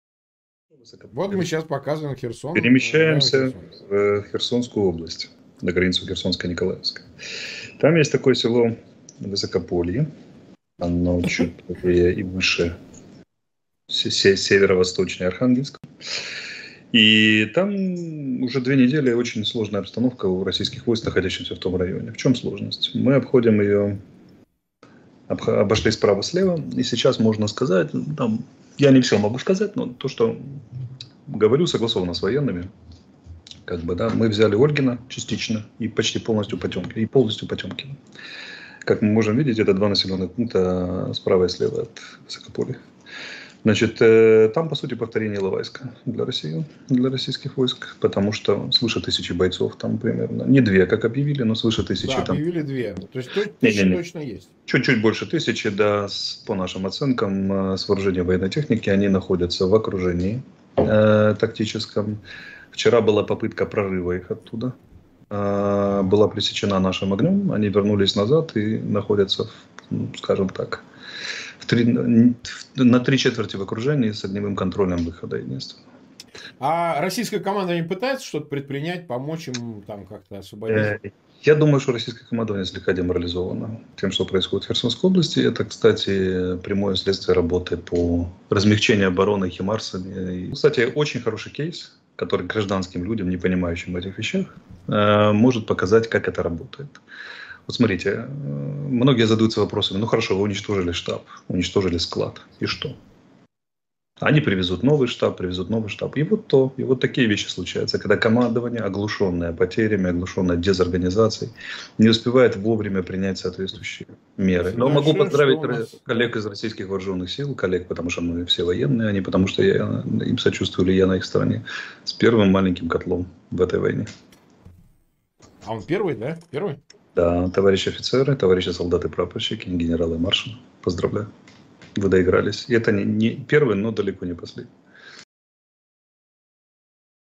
🔥🔥АРЕСТОВИЧ заявил про «котёл» в районе Херсонской обл.! Советник главы ОП назвал ключевые детали
🇺🇦Советник главы Офиса президента Украины Алексей Арестович сообщил о создании «котла» в районе Херсонской области для российских военных в населенном пункте Высокополье.